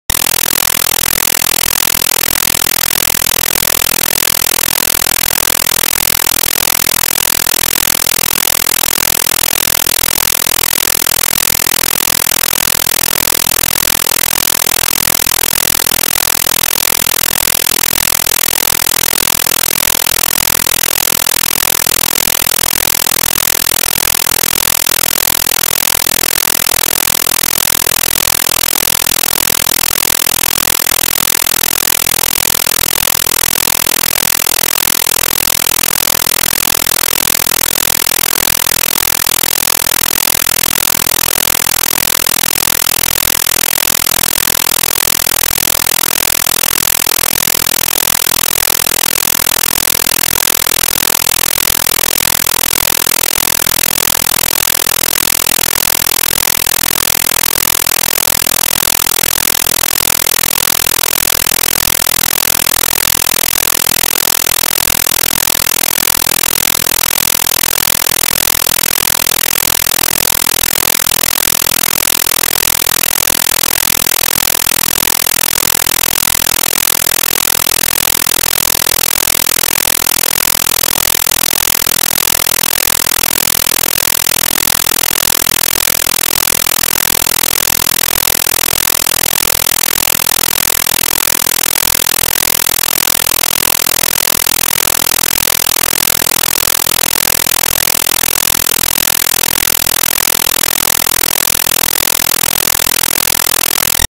ONLY RAJNI CAN-BULLET REBOUND TVS
This gag features Suresh Menon as 'Rajni' telling tales of his outlandish achievements.